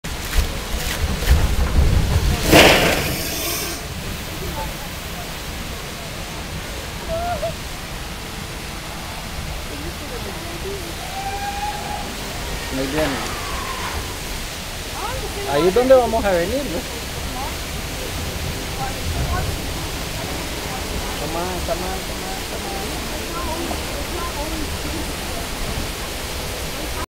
Splash Mountain por la Noche *
Flotando en Splash Mountain en Frontierland en Disney Magic Kingdom y en camino a la gran caida de agua de 5 pisos de altura.

Por cierto...¡ El agua estaba bien fría!  VID00739